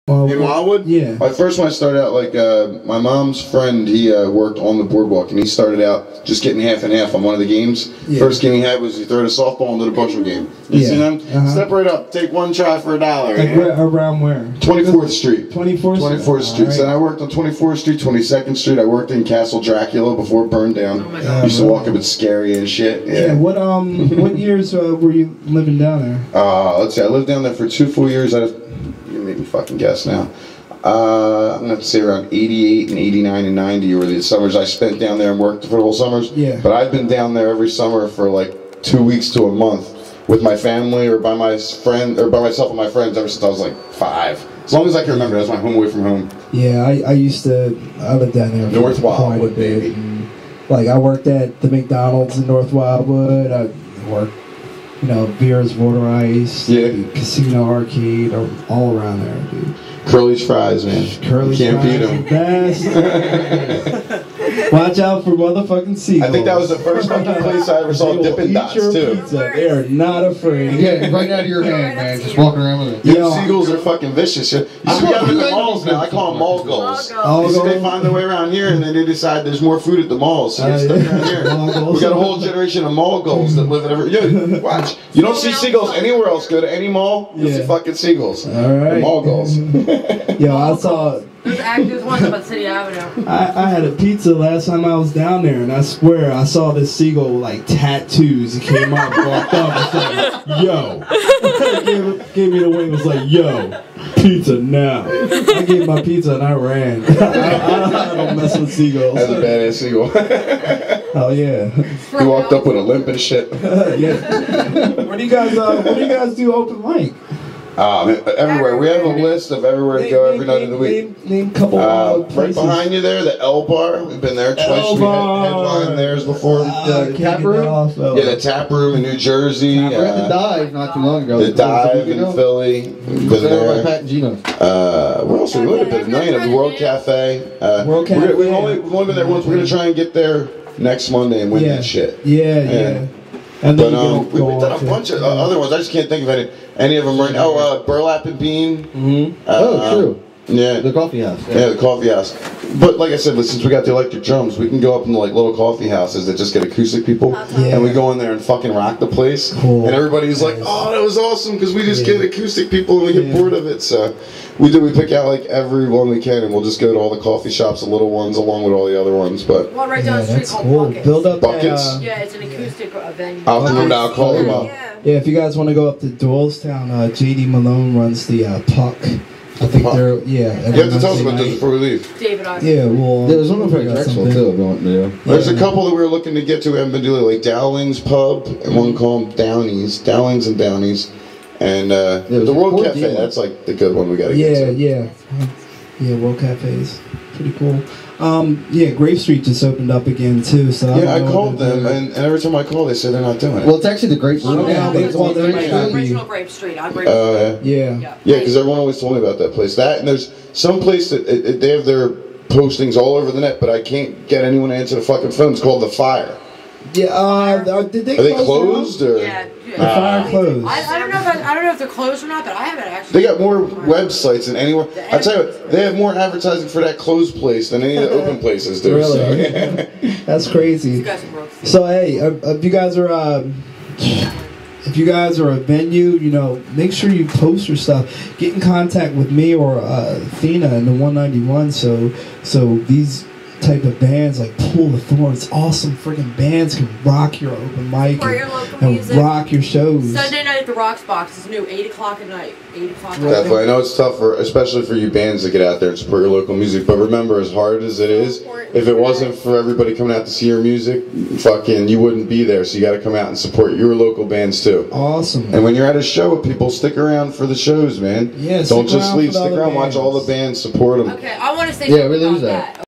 Pool of Thorns interview LIVE !!!! on web radio Indie191live
P.O.T.-  One of New Jersey/Philadelphia biggest upcoming bands. Quicly gaining the name "Deleware Valleys hardest working band" doing benefits, fundraisers and shows from Baltimore to New York.